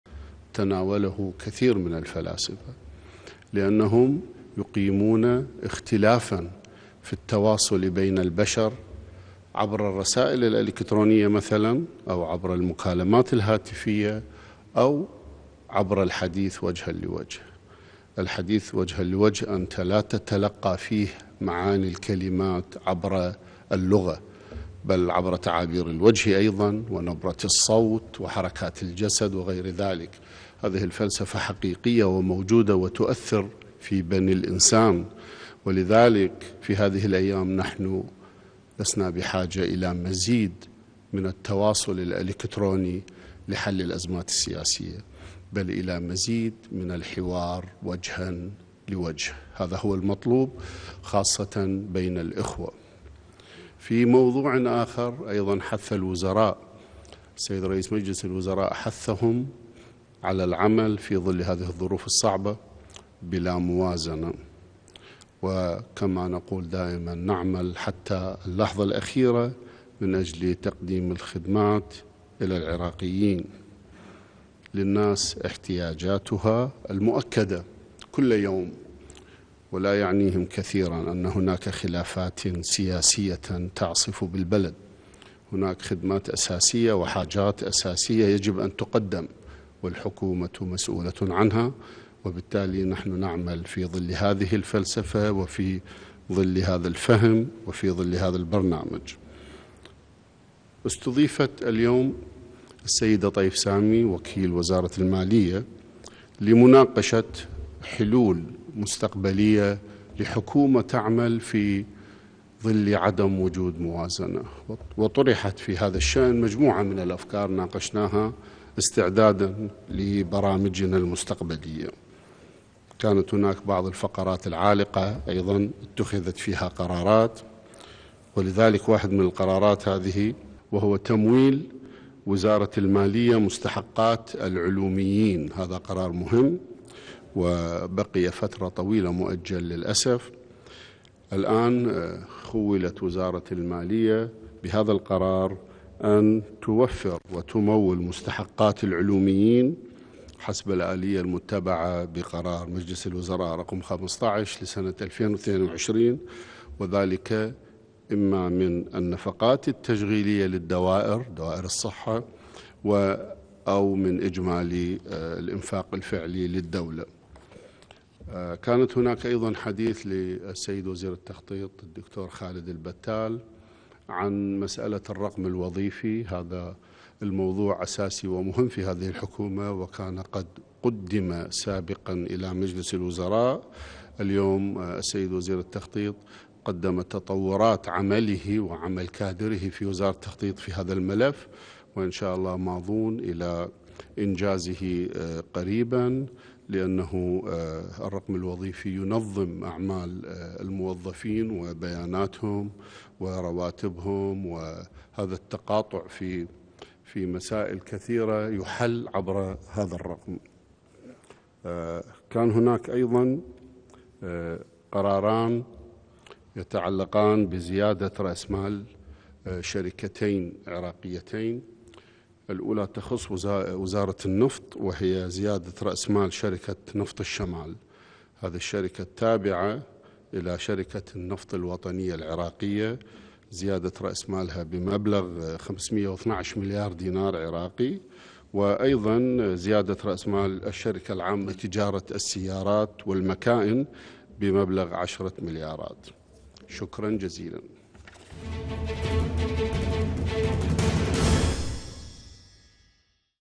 الايجاز الصحفي الاسبوعي للمتحدث باسم مجلس الوزراء حسن ناظم
حسن ناظم: مجلس الوزراء قرر تمويل مستحقات العلوميين
ناظم: ماضون بإنجاز مشروع الرقم الوظيفي الخاص بموظفي الدولة
ناظم: مجلس الوزراء استضاف وكيل وزارة المالية طيف سامي لمناقشة الحلول المستقبلية

موقع قناة التغيير الفضائية الالكتروني